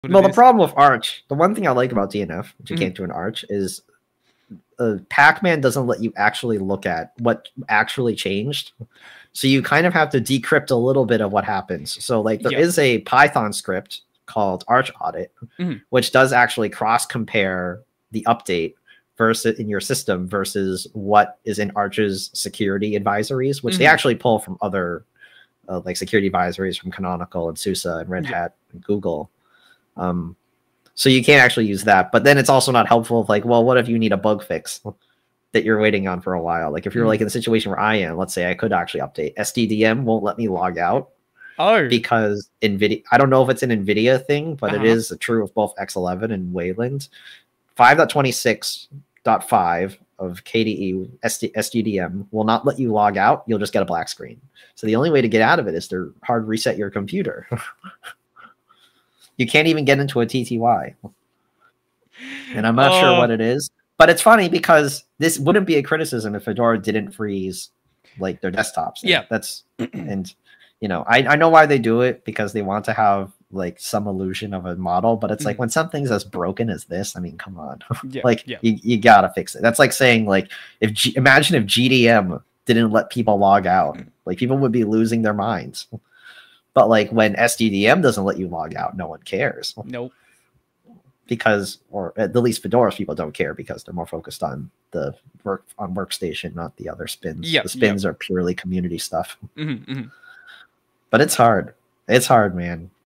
A Feature Every Linux Package Manager Needs
Linux is all about package managers but there is one really cool feature that a lot of them are annoyingly missing.